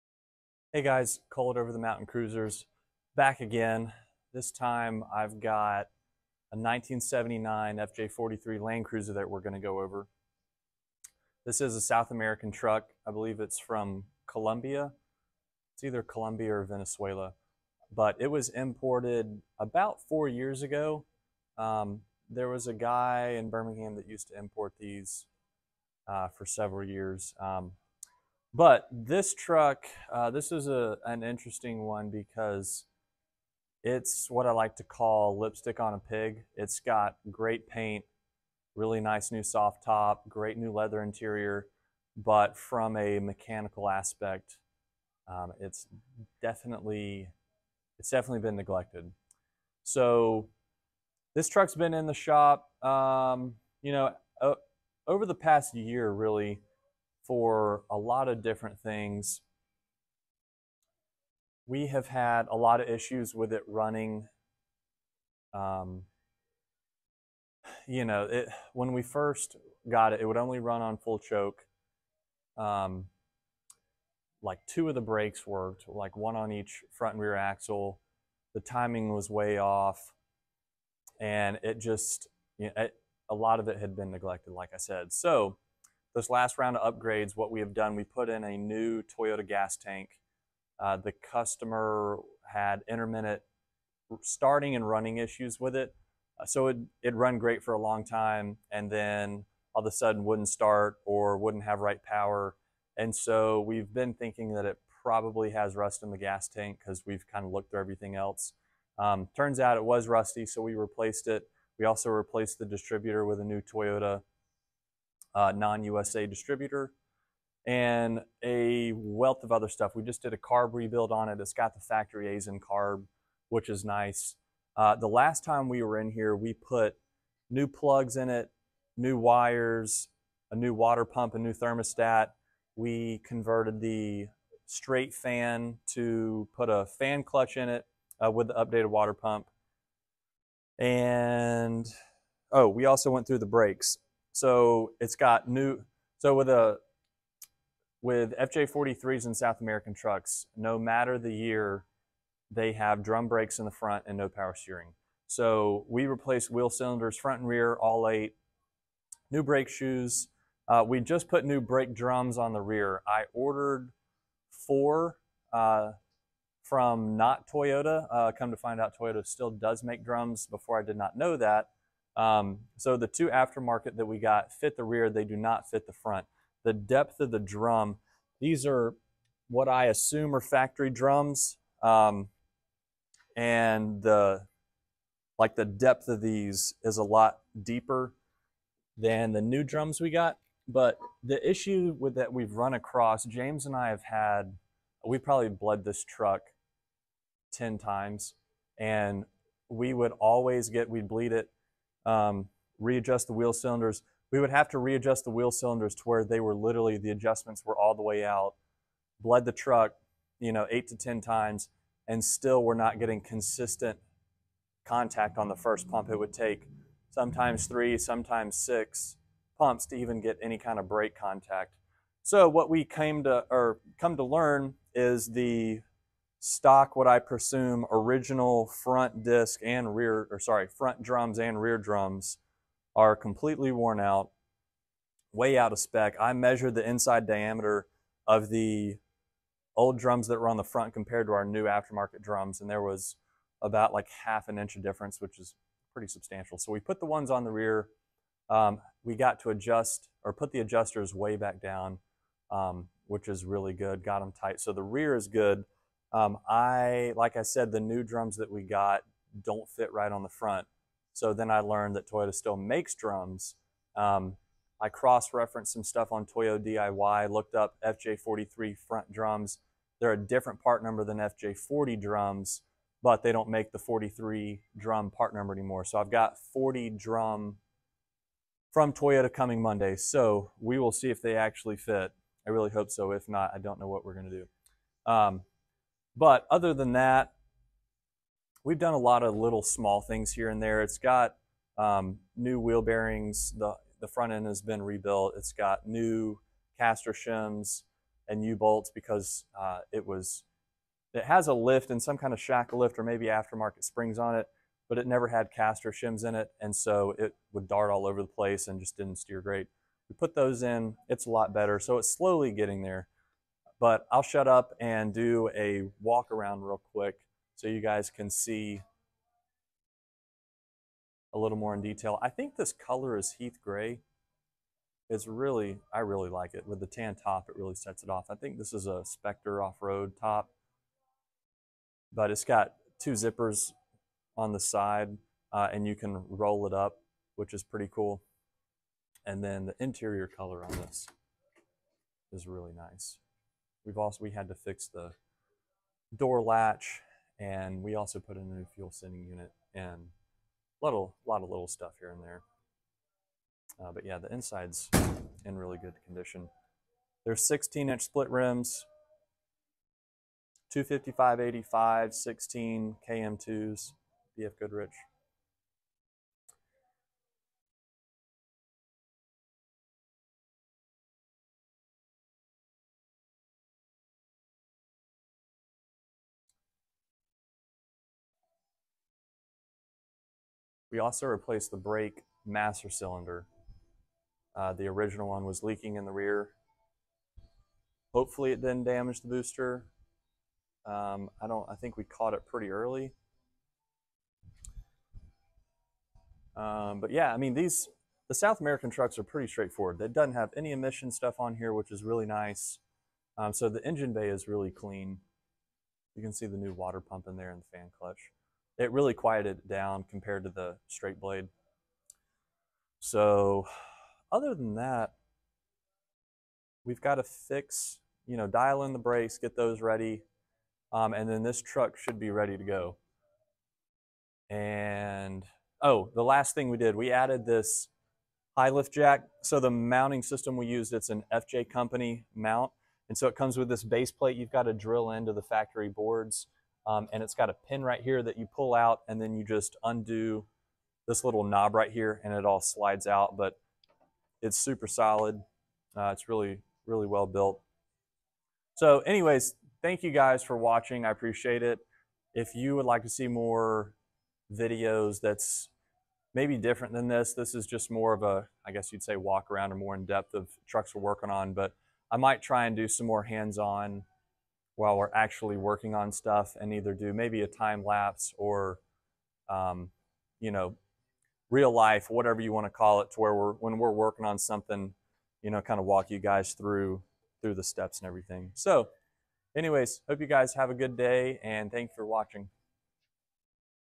OTM In the Shop - 1979 FJ43 Land Cruiser
A look around a South American FJ43